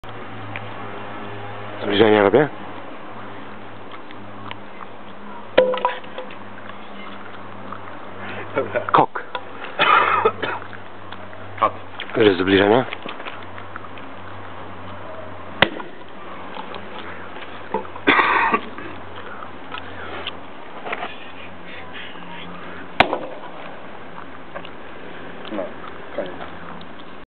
crash test motorola
bliskie spotkanie motoroli z siekierą